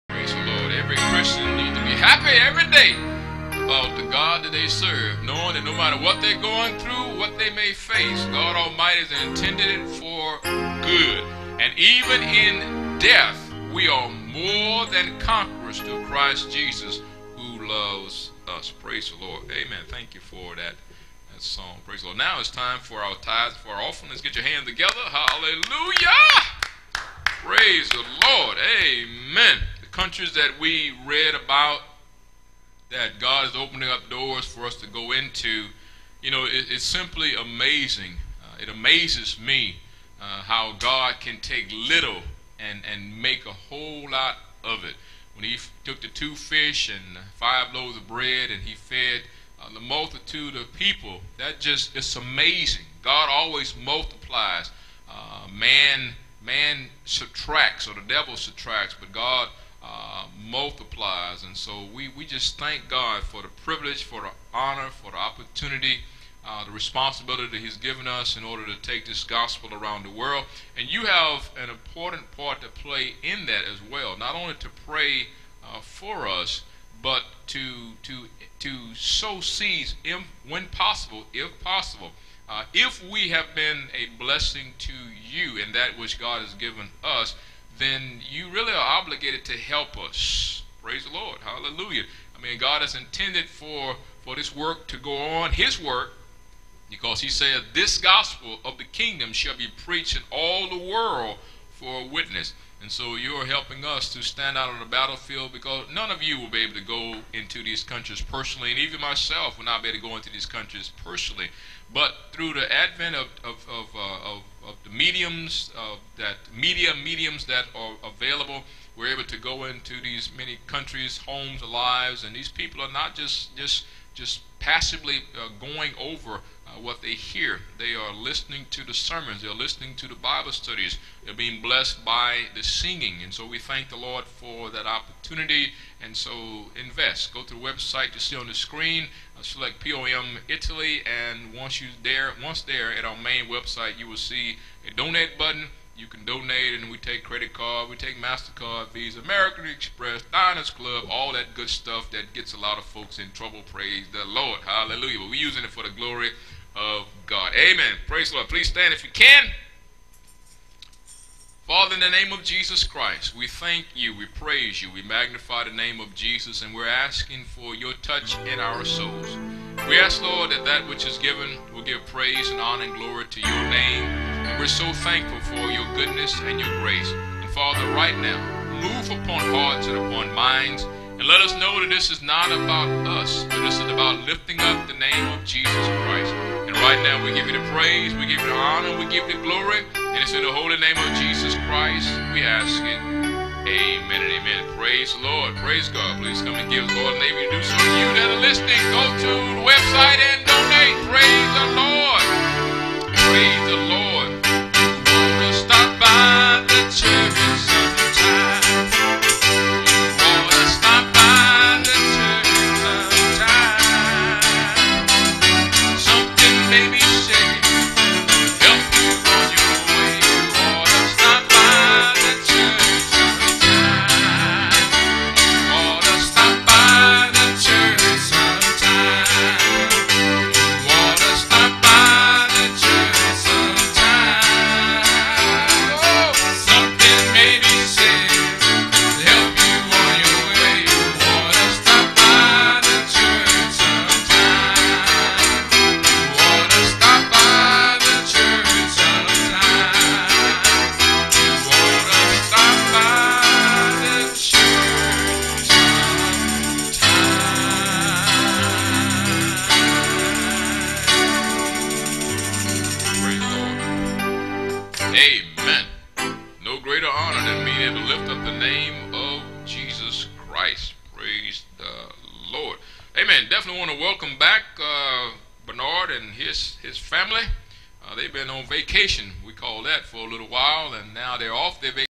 "The Fields are White to Harvest" (Tithes and Offerings: 23 Oct 11)
Please help us tell others who have not heard the message of the Cross.

So, why not be a greater BLESSING to others today?

Recorded "Live" at the Pilgrim Outreach Ministries headquarters church, Naples, Italy.

- Jesus is the Source, The Cross is the Means! -